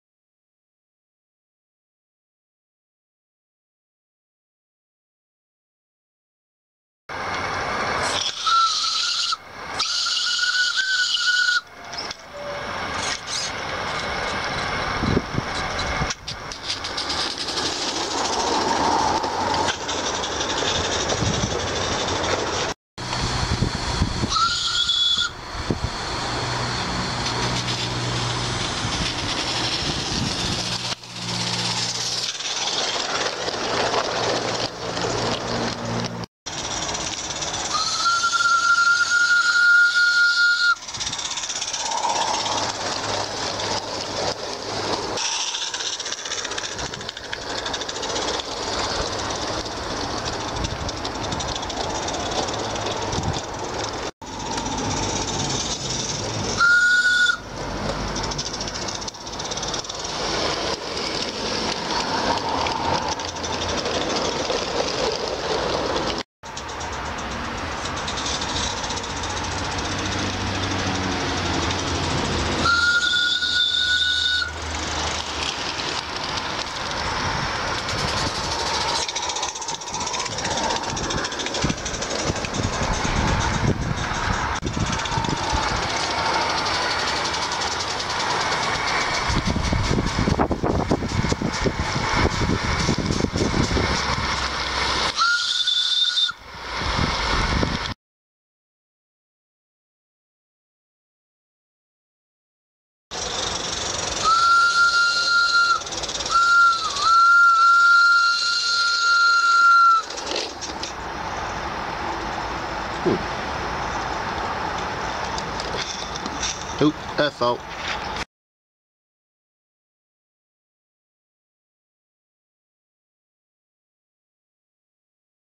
VoR in the garden
A Roundhouse VoR That came to me for a bit of TLC...